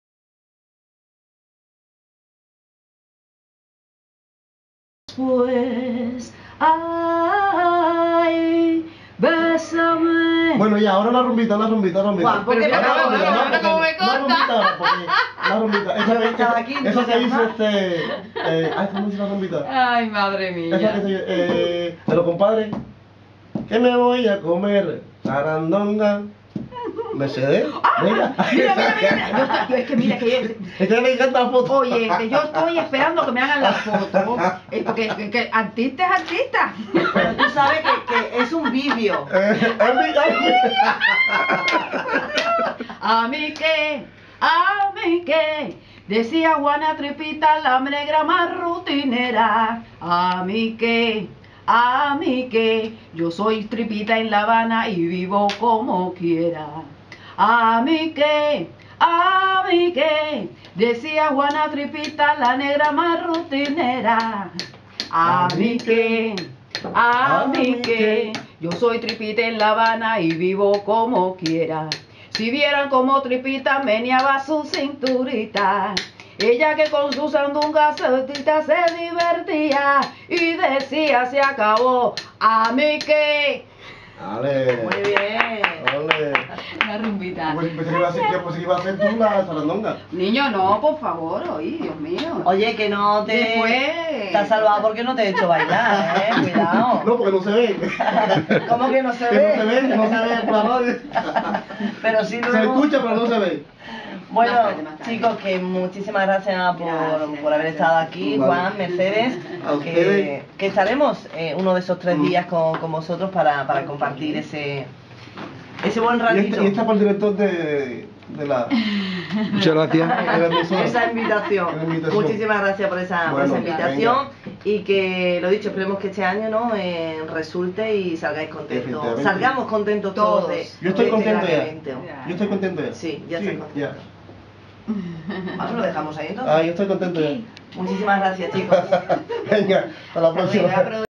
ENTREVISTA RADIO ARCOS VI ENCUENTRO " ARCOS BAILA 2012 "
UN MOMENTO DE LA ENTREVISTA DE MARIA JOSÉ LOZANO A JUAN ARMANT Y MERCEDES RISET, SOBRE EL VI ENCUENTRO "ARCOS BAILA" A CELEBRARSE EL PRÓXIMO FIN DE SEMANA 20,21 Y 22 ABRIL EN EL HOTEL EL LAGO.